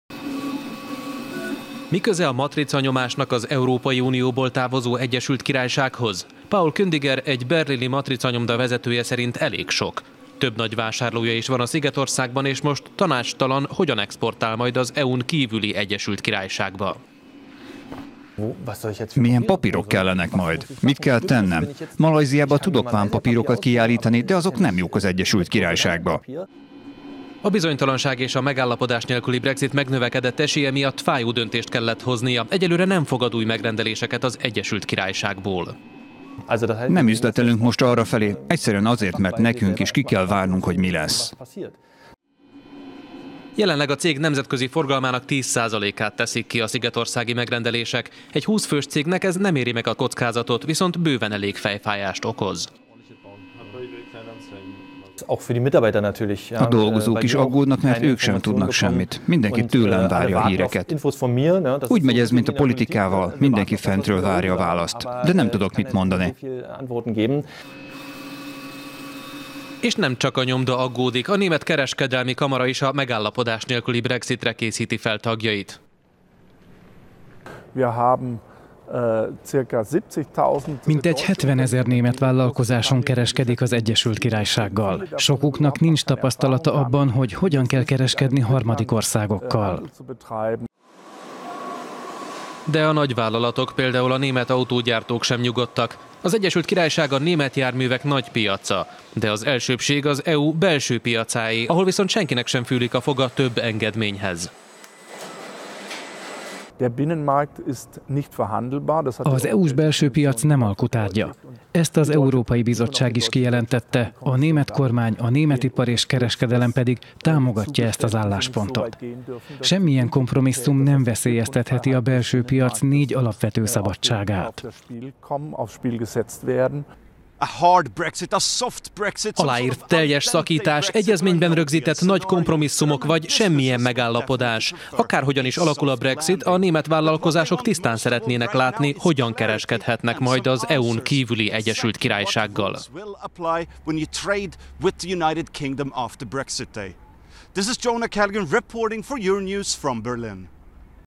Brexit: a német cégek is válaszokat várnak
Nem tudják a német vállalkozások, hogyan kereskedhetnek majd az EU-n kívüli Egyesült Királysággal.…